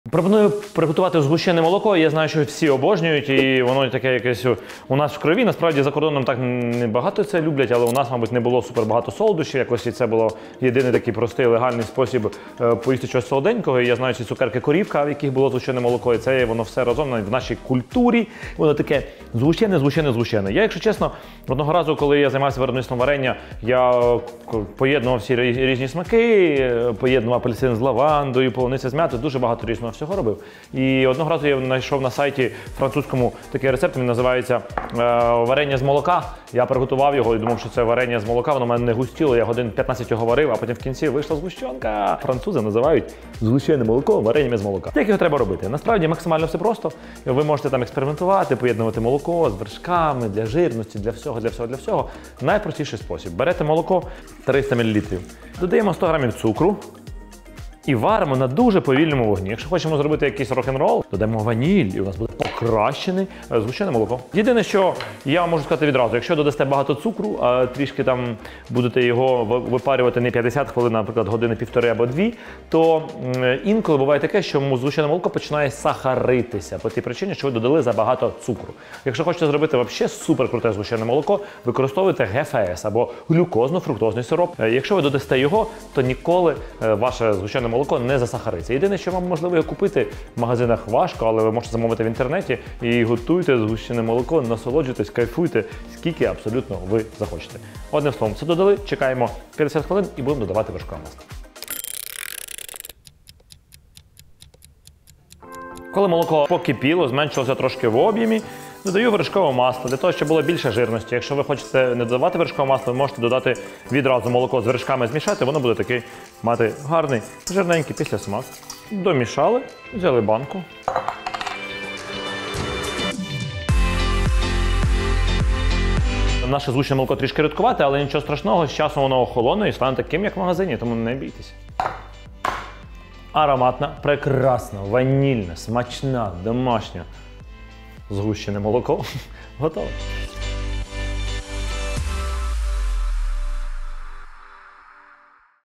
ЗГУЩЕНЕ молоко рецепт - всього ТРИ інгредієнти | Поради Євгена Клопотенка
Згущене молоко з всього 3 компонентів. Навчу вас робити цей смаколик за кілька простих кроків.

Інгредієнти для приготування згущеного молока:
300 мл молока 3,2 % жирності
100 г цукру
10 г ваніліну (за бажанням)
30 г вершкового масла

Детально, як приготувати згущене молоко в домашніх умовах дивіться у відео.

⏱ Time Code:

0:00 - Готуємо згущене молоко
0:25 - Що таке варення з молока
0:50 - Рецепт приготування згущеного молока
1:07 - Чому згущенка цукориться
1:20 - Для чого використовують ГФС
1:39 - Скільки варити згущене молоко
2:08 - Коли загусне згущене молоко
2:12 - Презентація готової страви

Я та моя команда працюємо, щоб допомагати Україні.

Поширюйте і рекомендуйте відео. Це величезний вклад в розвиток українського контенту!